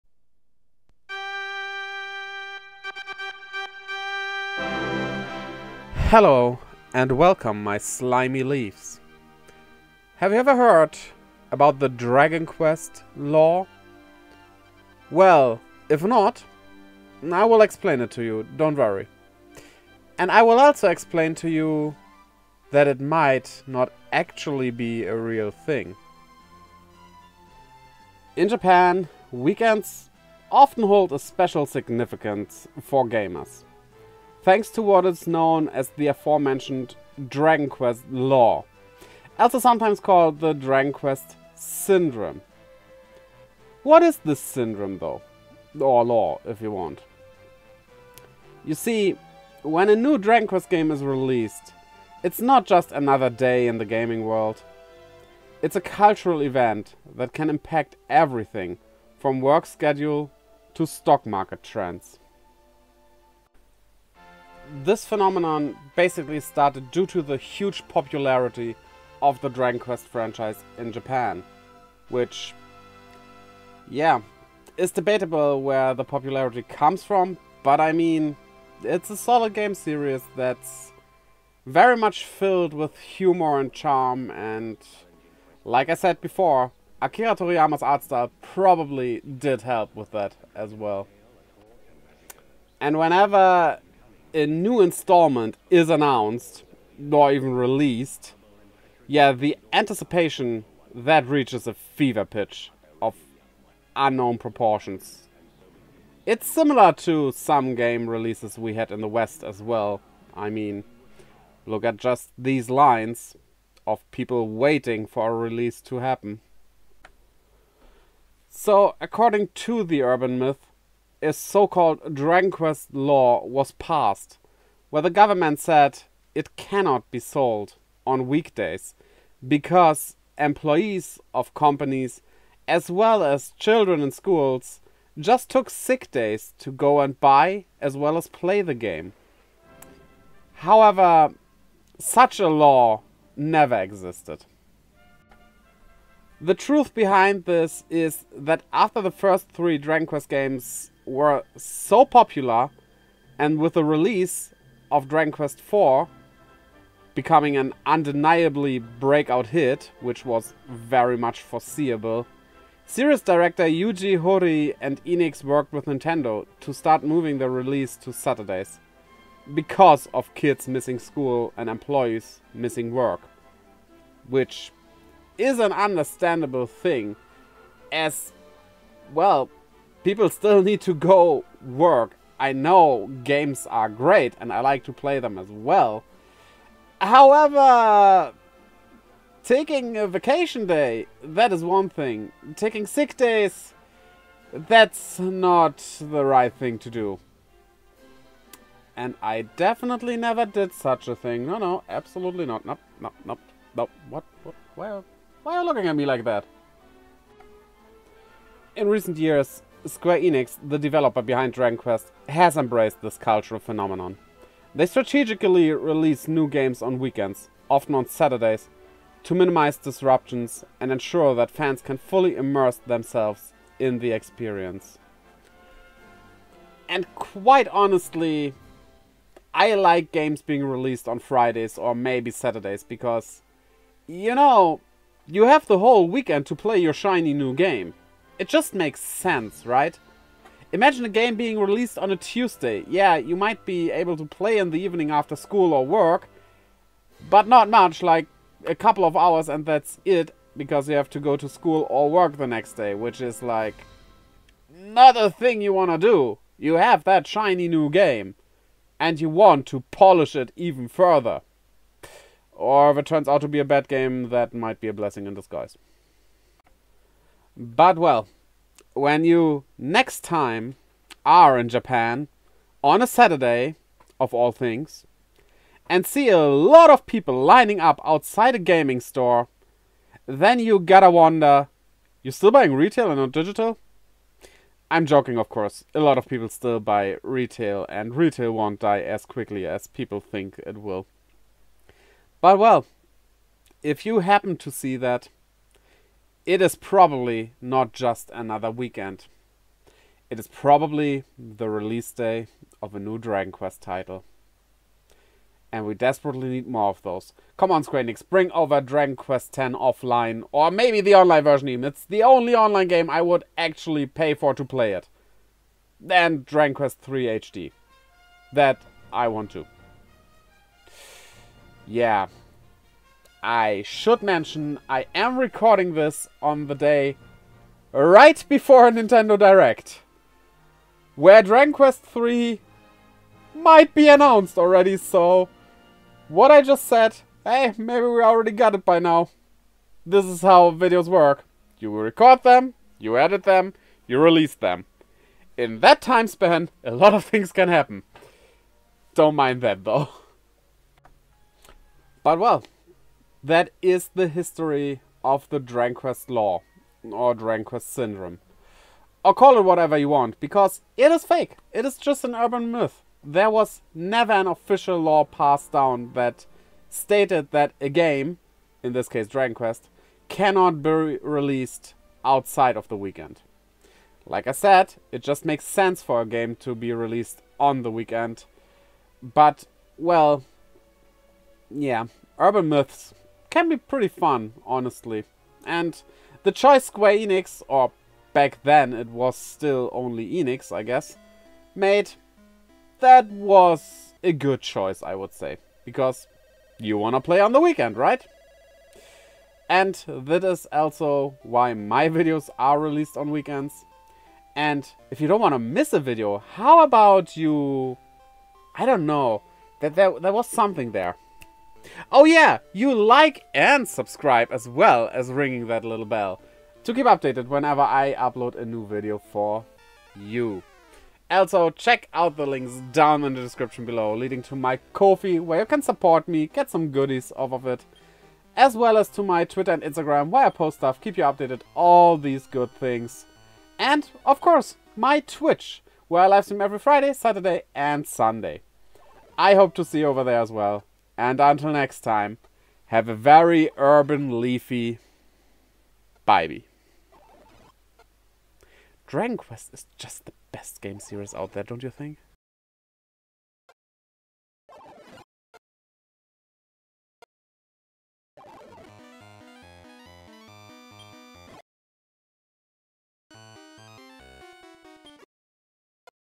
The TRUTH about the DRAGON QUEST LAW! | Rief the Leaf
So, is there actually a Dragon Quest Law in Japan? The answer can be found here.

Visit me on Twitch, Live every Friday, Saturday, Sunday and Monday at 11am PDT, 2pm EDT, 8pm CEST